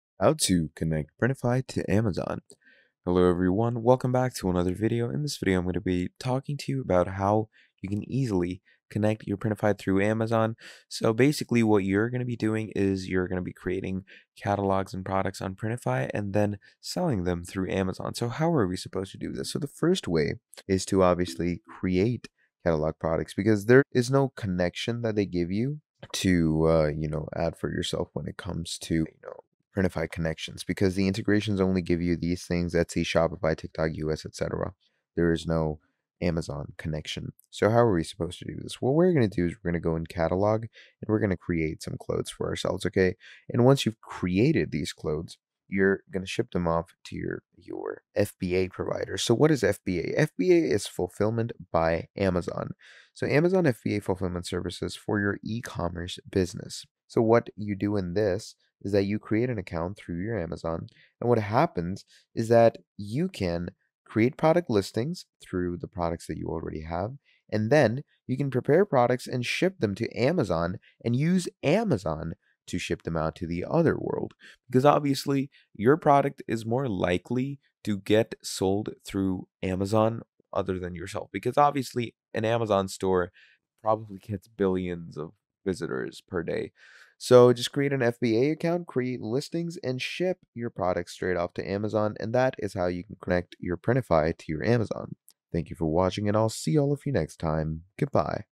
How To Connect Printify To Amazon (Step By Step Guide)
in this video I will show you How To Connect Printify To Amazon. This video will cover everything that you need to know and help you get started with this software.

Kind regards,